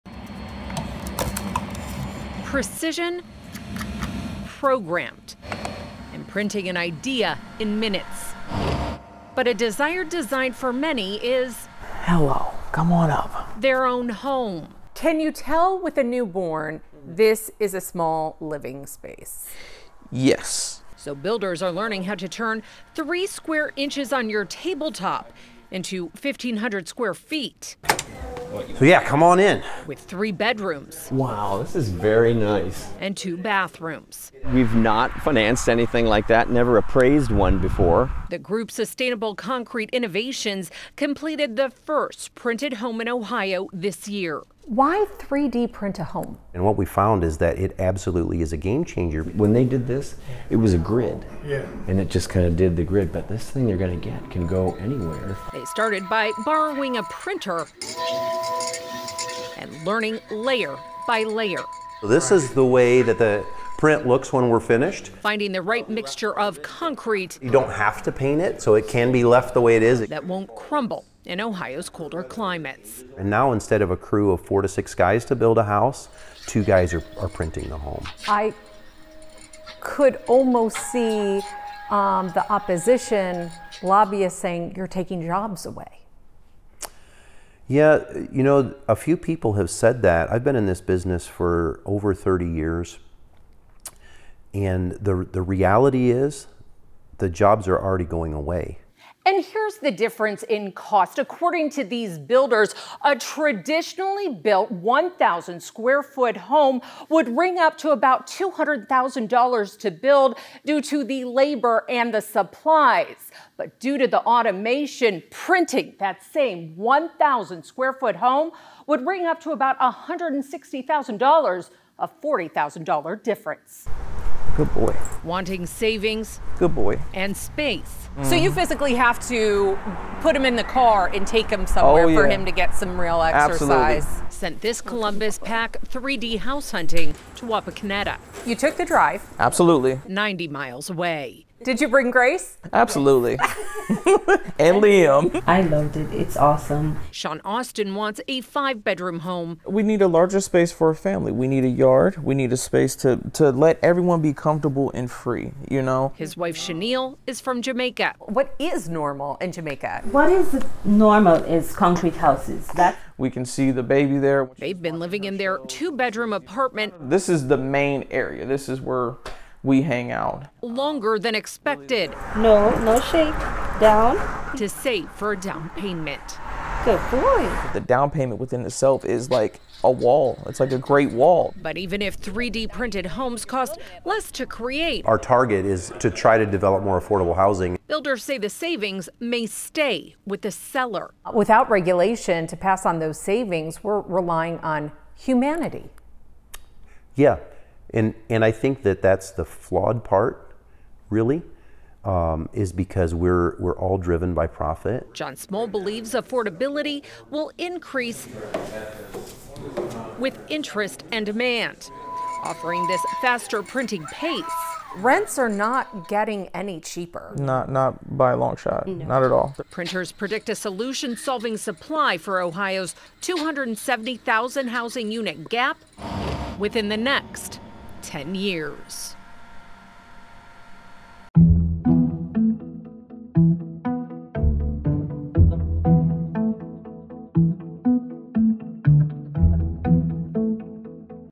Builder of first Ohio 3D-printed home says own 'for the price of a car payment'
Building a home in less than a week for a fraction of the cost may sound unrealistic. That's why Sustainable Concrete Innovations out of Wapakoneta, Ohio, held an open house this summer to allow industry professionals to tour the first completed 3D-printed home in the state.